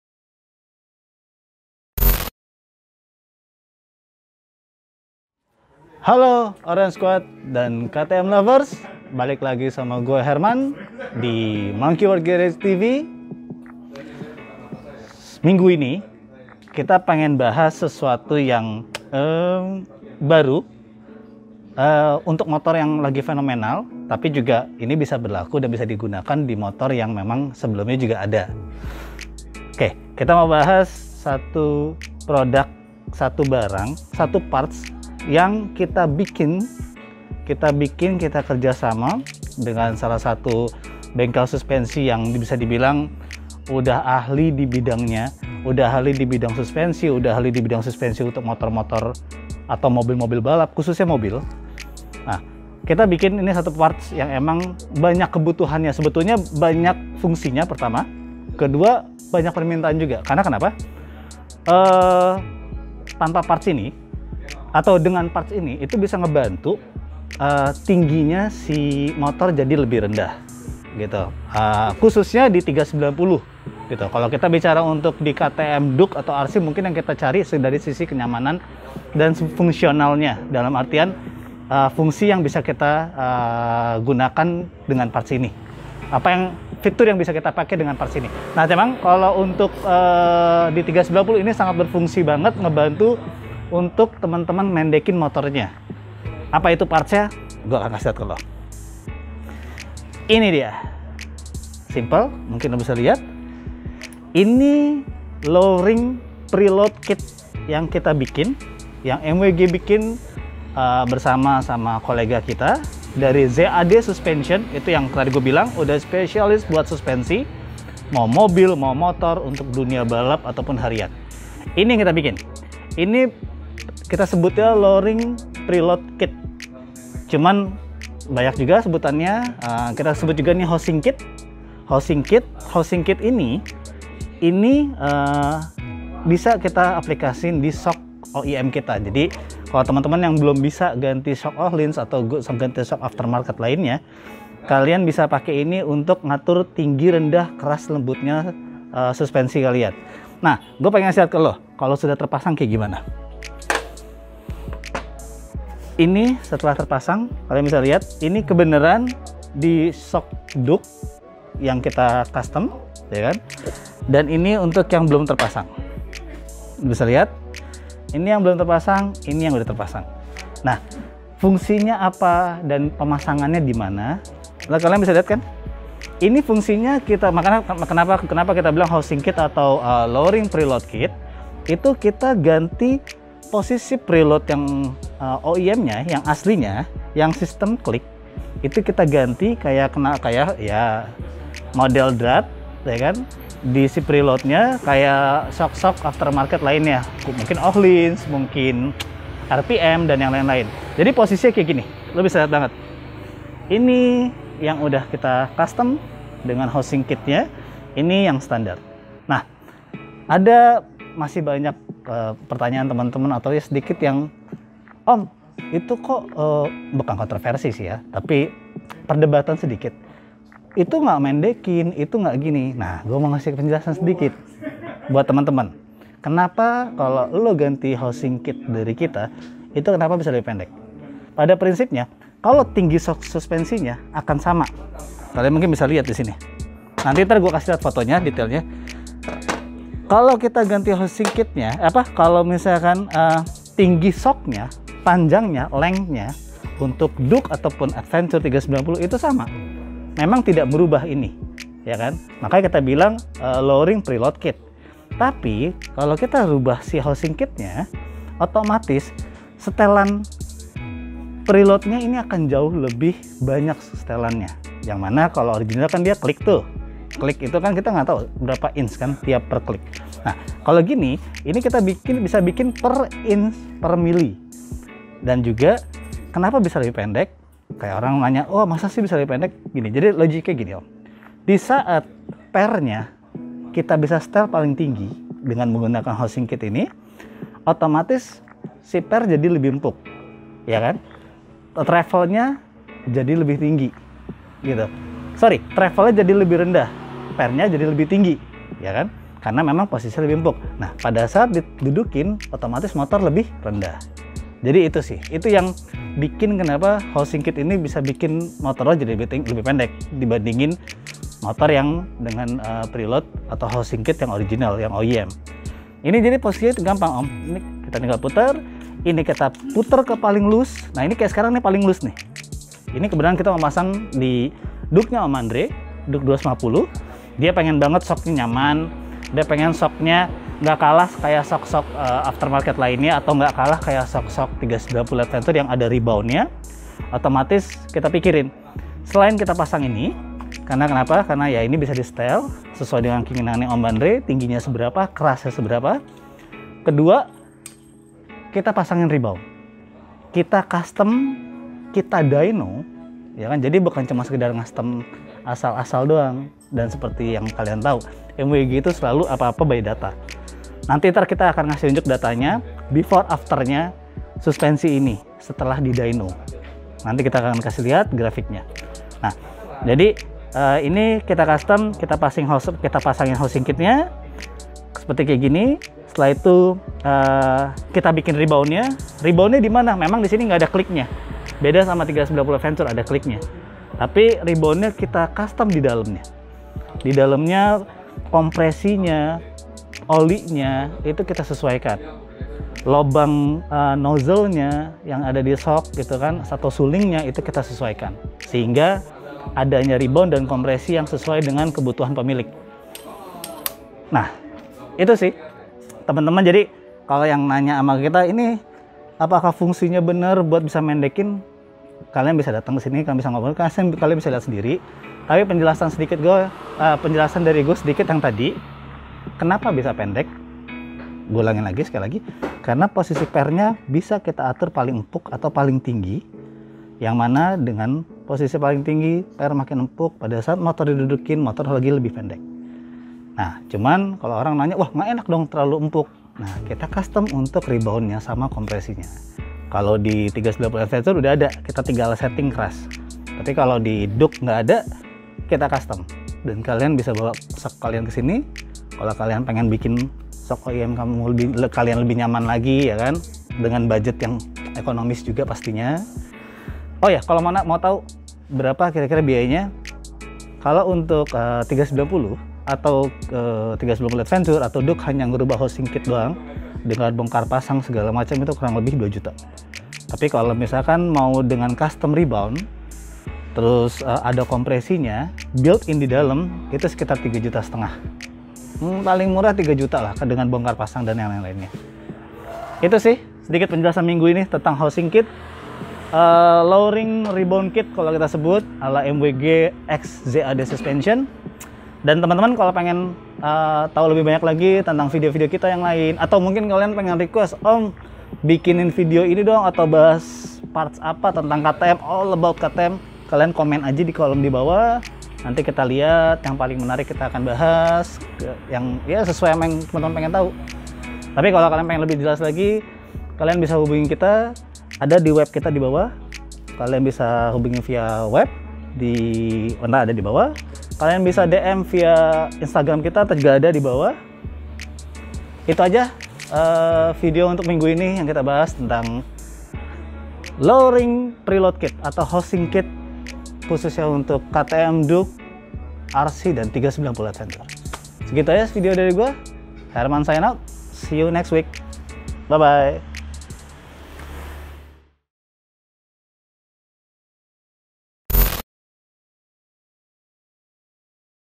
Suspensi Bawaan KTM Dipakein Housing Kit/Lowering Kit! Nambah Rebound, Preload, OEM Rasa Solihin!
Orange Squad n MWG Squad ingin punya Solihin? namun dompet berteriak Tidak seketika? atau Orange Squad n MWG Squad ingin riding dengan motor kalian tanpa jinjit-jinjit manis? Tenang saja, kini MWG sudah bisa mengcustom Shock bawaan kalian sehingga mempunyai Rebound n Lowering kit Sekaligus!

Catalog: @mwg_catalog

Tokopedia/monkeyworkgarage
Bukalapak/monkeyworkgarage

MONKEYWORK GARAGE
Powerpart Custompart  Accessoires
KTM DUKE RC ADV SPECIALIST